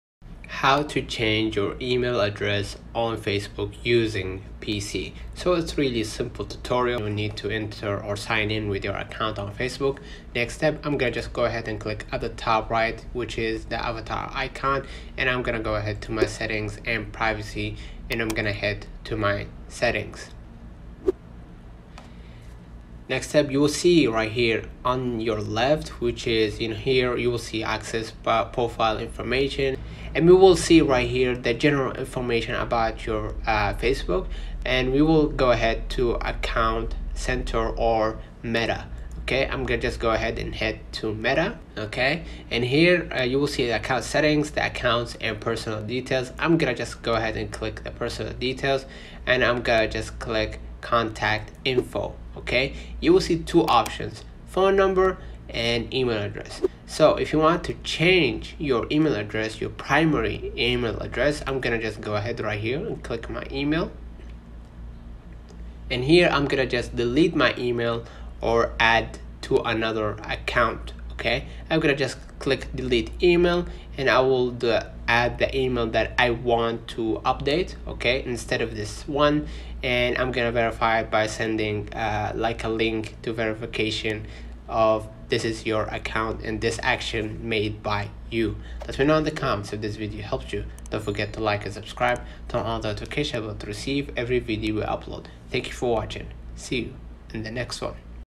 How to change your email address on Facebook using PC (FULL GUIDE)
You'll learn how to change your email on facebook pc. Basically, how to change your email on facebook 2024. Enjoy!